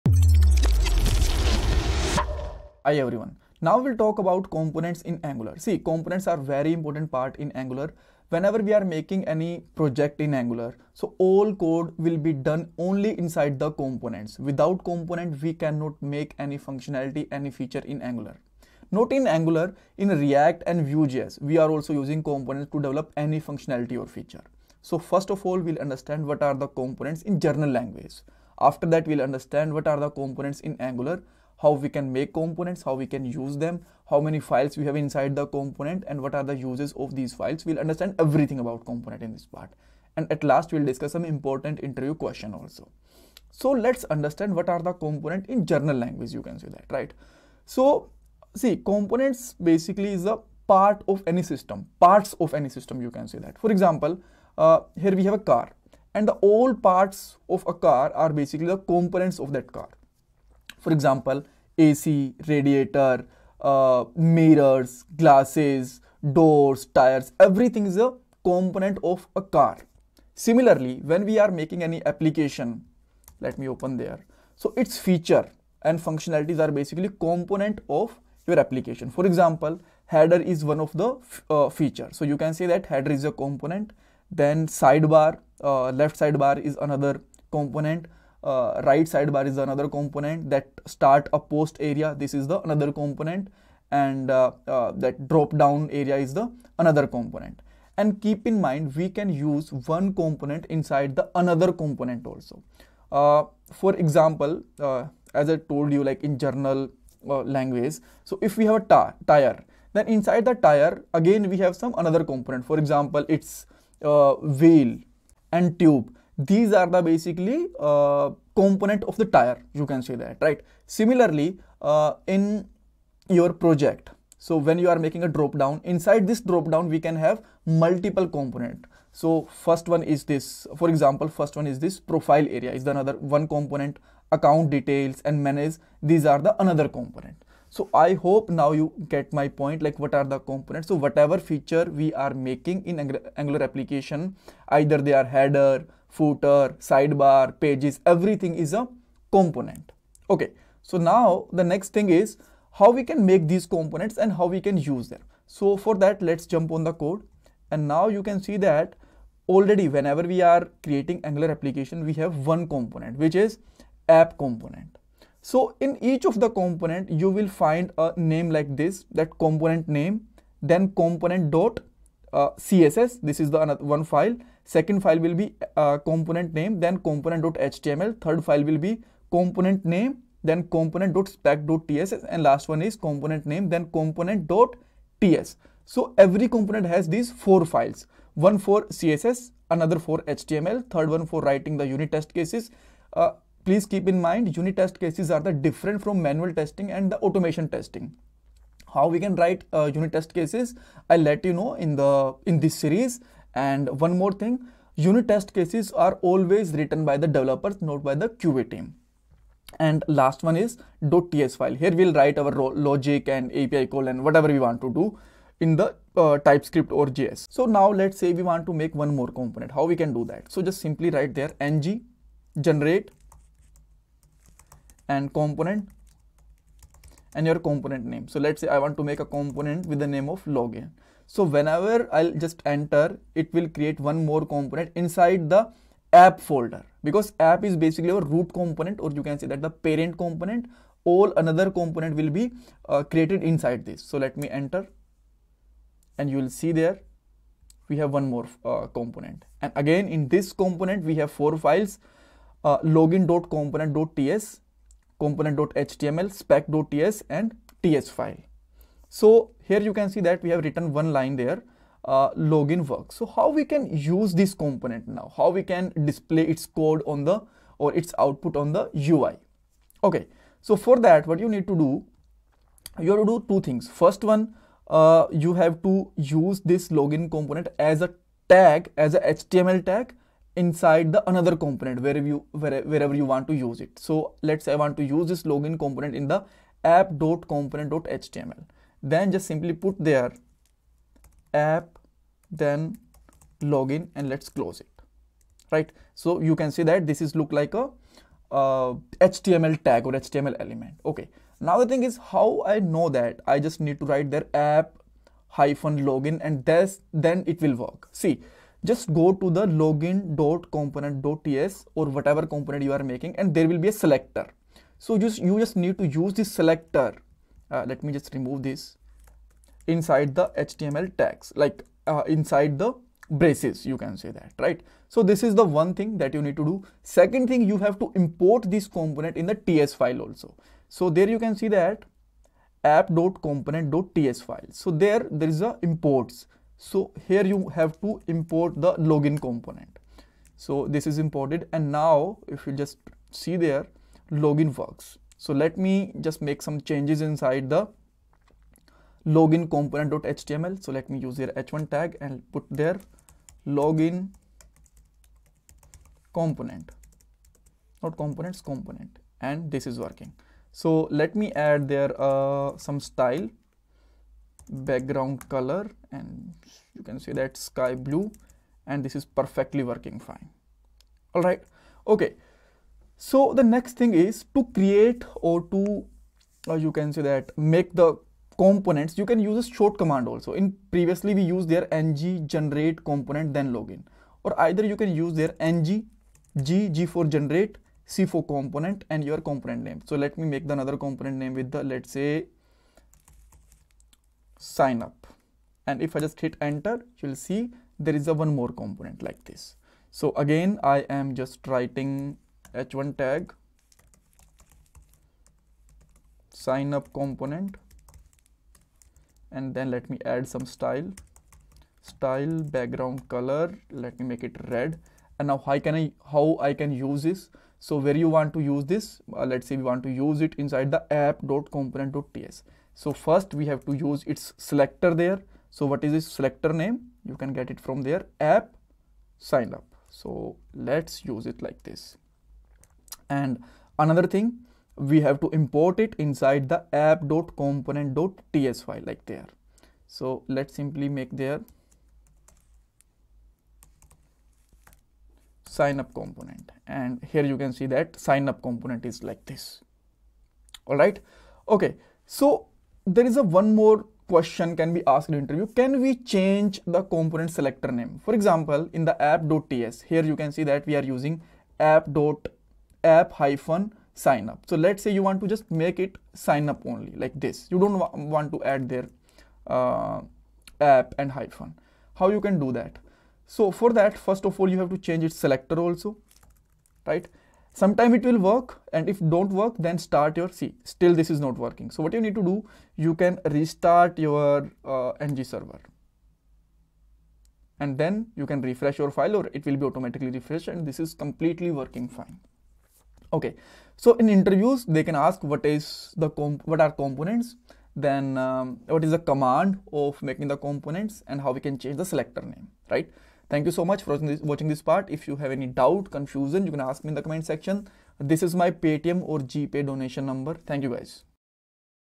Angular 19 Tutorial #7 Component
In this video, we'll delve into one of the fundamental building blocks of Angular applications: Components.

We'll cover the following:
What is Component
What is Component in Angular
How to make  and use component
Files of component
Interview Questions
Files of component
Interview Questions
What is a Component?
A basic understanding of Angular components
How components are used to build user interfaces
Component Structure:
Breaking down the key parts of a component:
Template
Styles
TypeScript class

Create and use Angular components
Understand the role of components in Angular applications
Leverage component lifecycle hooks to optimize your application's performance